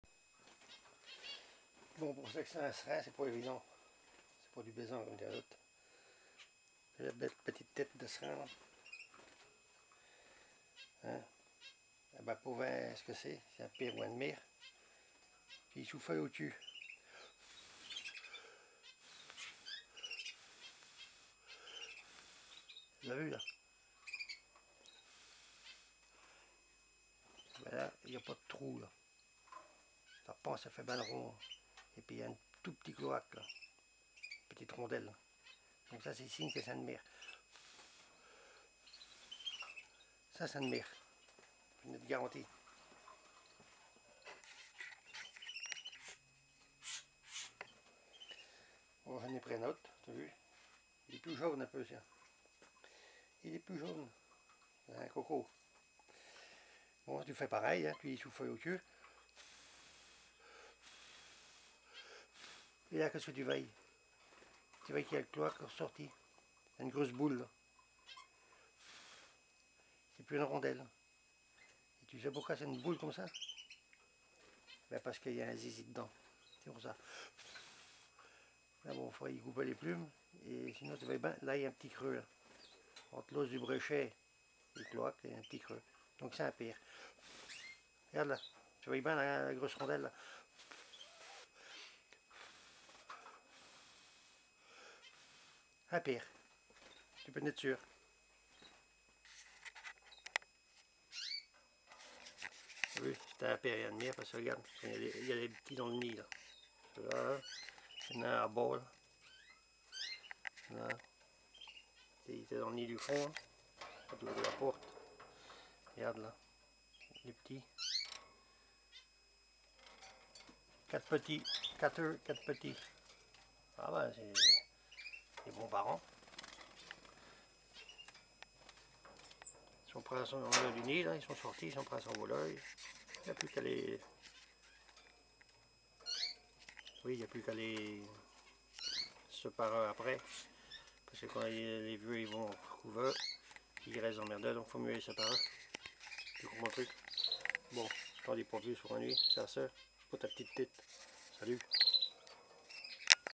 sexage de serin.AVI
sexer des serins ou des canaris n'est pas chose facile LaBouine vous donne quelques explications